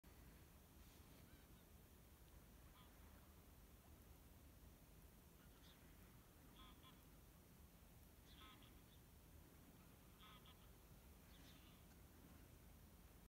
October sunset, Graemsay
A calm evening sunset on Graemsay, Orkney Islands